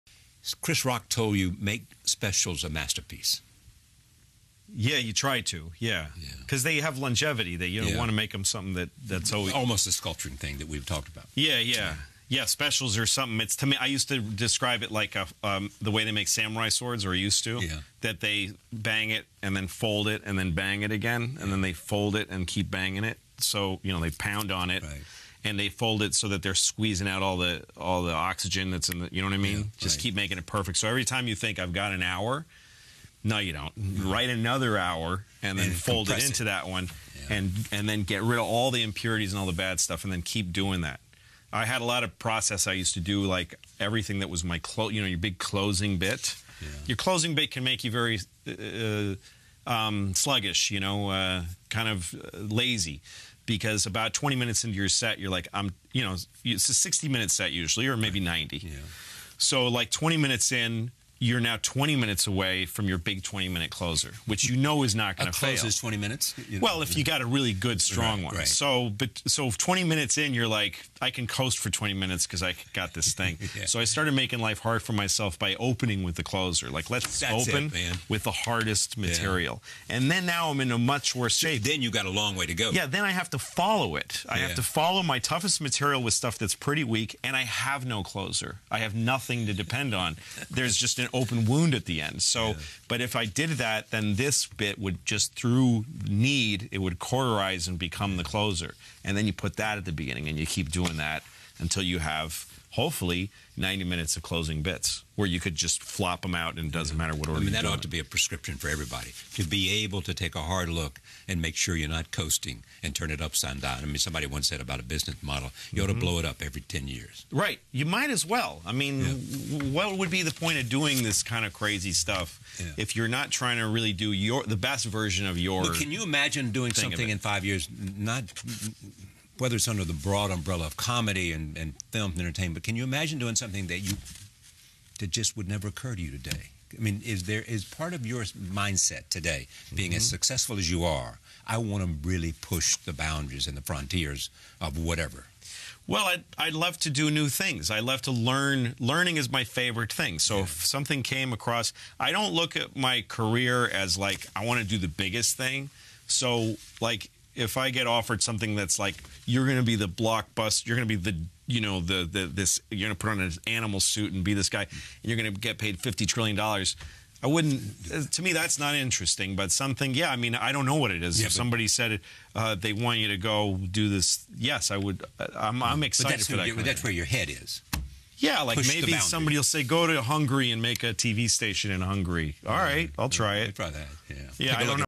Louis CK - Learning Is My Favorite Thing
Louie talks about his approach to stand-up, and how comedians often get lazy once they have a closing act that is reliable. It's like anything that you're good at -- you can become complacent. He talks about how he got around any potential pitfalls from being complacent and talks about his approach to career and success -- to be always learning, always moving.